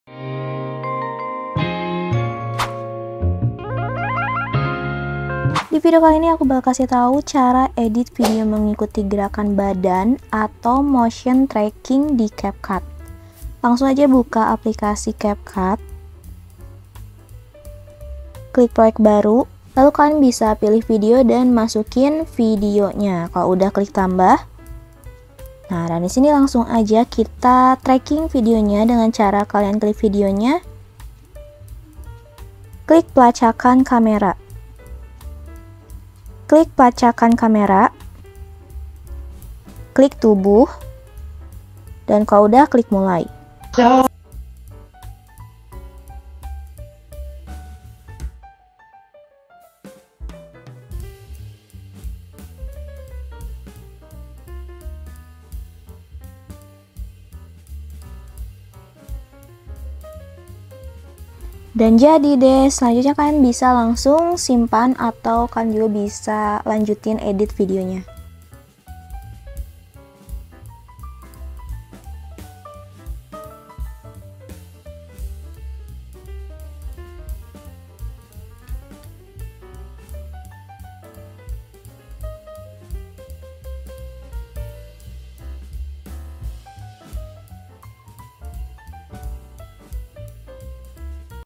Edit video mengikuti gerakan badan capcut motion tracking
Video kali ini membahas tentang tutorial capcut motion tracking atau mengedit video mengikuti gerakan badan.